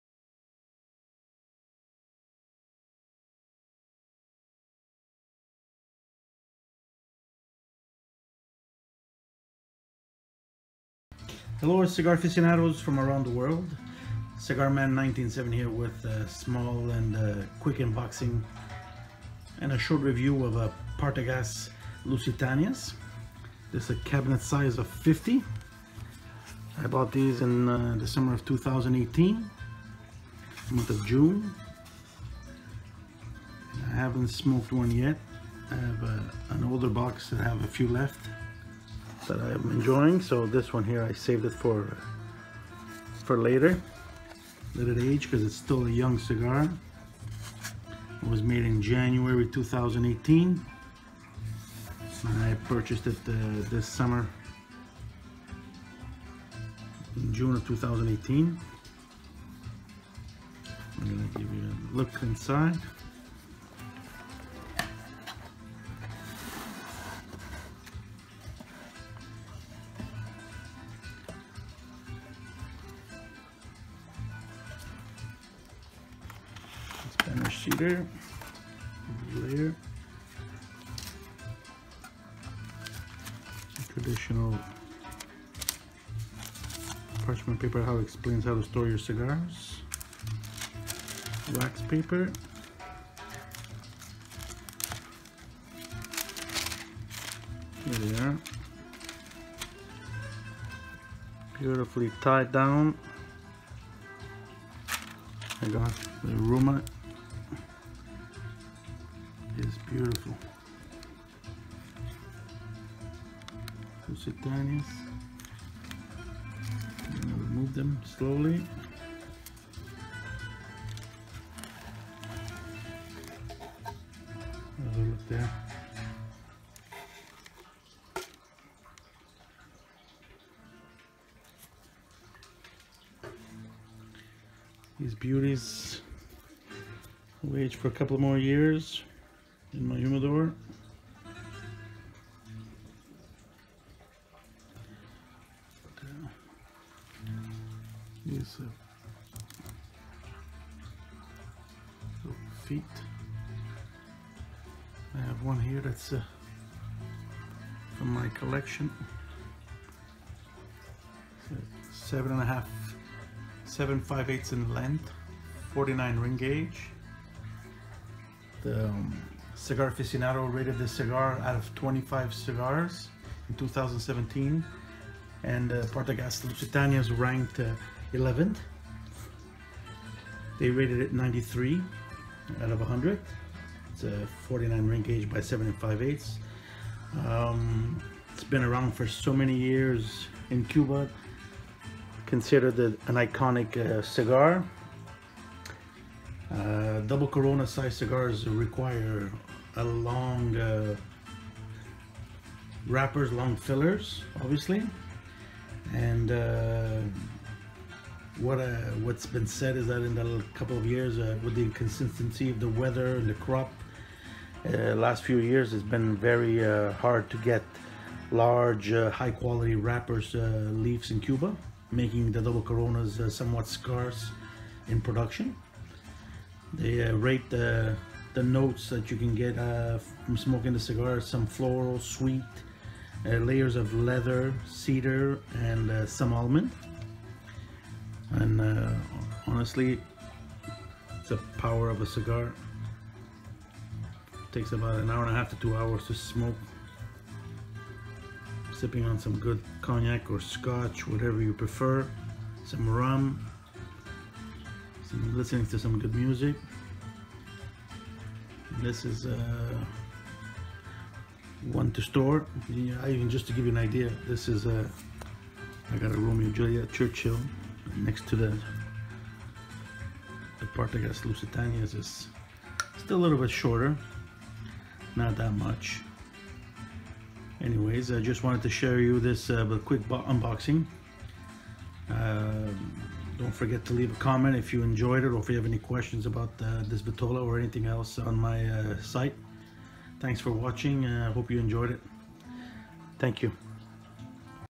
🇨🇺 CUBAN CIGAR - PARTAGAS LUSITANIAS HUGE CABINET of 50 cigars WOW!!
A quick look at this HUGE & RARE cabinet of 50 Lusitanias.
I bought these in cuba ,summer of 2018 . The cabinet was made in January 2018.
As a Canadian tourist we are allowed only 50 cigars, which I obtained with this one single purchase LOL.